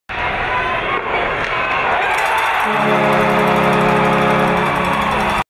Seawolves vs. Lake Superior State
Remember Brett Cameron's game-winning goal vs. Lake Superior State in December - with 16 seconds left in regulation?

Catch the Seawolves and Lakers, Feb. 7-8 on America ONE Sports, while Friday's game will be shown live at Crossbar at 3:30 p.m. — with Brett Cameron at Sullivan Arena.